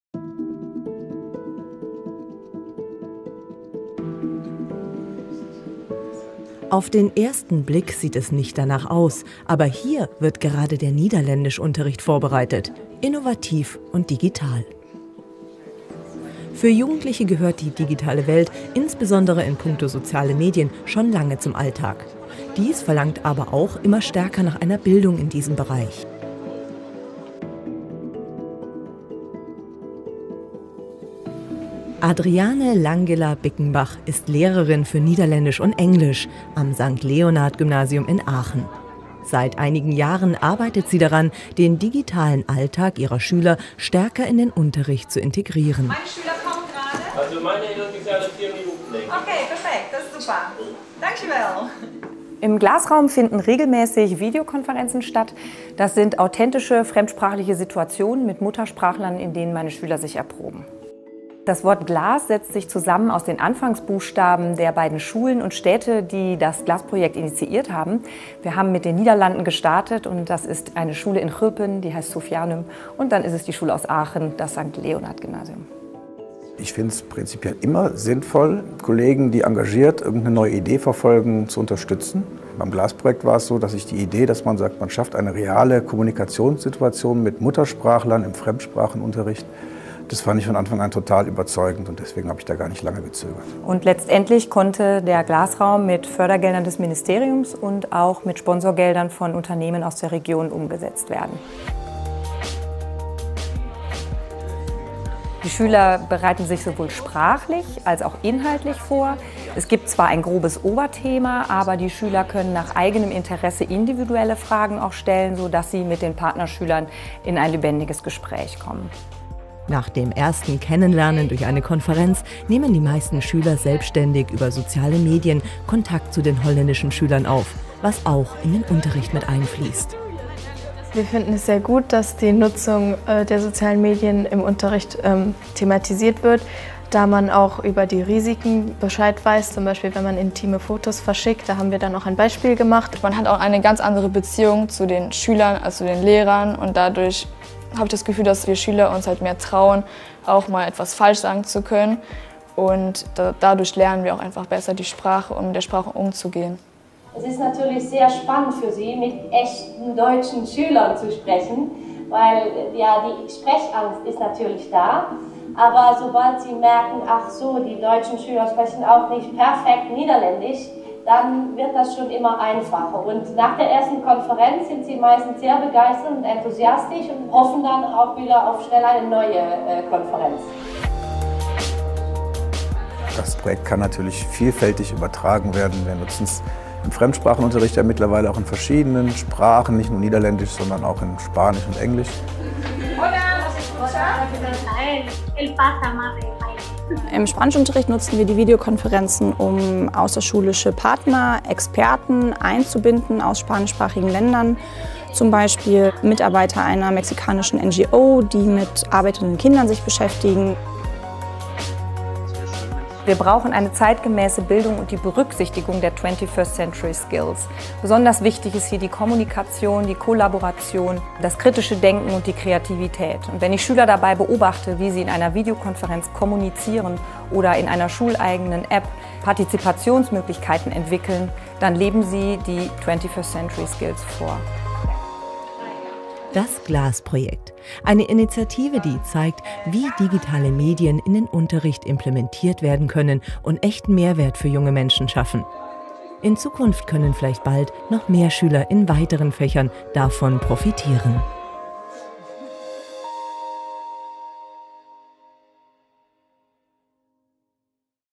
Deutscher Lehrerpreis 2017, 1. Preis Kategorie "Unterricht innovativ"
Team "GLAS-Projekt", St. Leonhard Gymnasium, Aachen
Das St. Leonhard Gymnasium in Aachen hat sich der Herausforderung gestellt, eine moderne, authentische, nachhaltige Kommunikation zu entwickeln. Durch das so genannte GLAS-Projekt erhalten Schülerinnen und Schüler die Möglichkeit, sich im Rahmen des regulären Fachunterrichts durch regelmäßige Videokonferenzen mit gleichaltrigen Teilneh- merinnen und Teilnehmern aus der niederländischen Partnerschule „Scholengemeenschap Sophianum“ in ihren jeweiligen Muttersprachen zu unterhalten. Der Name des Projekts setzt sich aus den Anfangsbuchstaben der beteiligten Orte und Schulen zusammen: G(ulpen), L(eonhard), A(achen), S(ophianum). Die Videokonferenzen finden in einem speziell ange- fertigten GLAS-Raum statt.
Der Fokus liegt auf der Umsetzung einer bereits bestehenden Schulpartnerschaft, die neben gegenseitigen Besuchen von einem regelmäßigen, digital basierten Austausch profitiert. Die erste Vorbereitung auf ein persönliches Treffen erfolgt durch E-Mail-Kontakt, eine lehrer- begleitete Videokonferenz und die selbstständige Nutzung der sozialen Netzwerke. Für die Vorbereitung der Videokonferenzen werden die Schülerinnen und Schüler beider beteiligter Schulen in Expertengruppen eingeteilt, die meist aus vier bis fünf Personen des etwa gleichen Kompetenzniveaus bestehen. Sie spezialisieren sich in diesen Lerngruppen auf Themenbereiche aus ihrer eigenen Lebenswirklichkeit. Die jeweiligen Untersuchungs- gegenstände werden dann am Ende einer Lerneinheit in einem authentischen Kontext mit Muttersprachlern aus den Niederlanden bzw. Deutschland angewandt und ermöglichen einen individuellen Lernzuwachs. Oberstes Ziel des Projekts ist die Erhöhung der interkulturellen Handlungsfähigkeit der Lernenden und die Steigerung ihrer fremdsprachlichen, kommunikativen und medialen Kompetenz.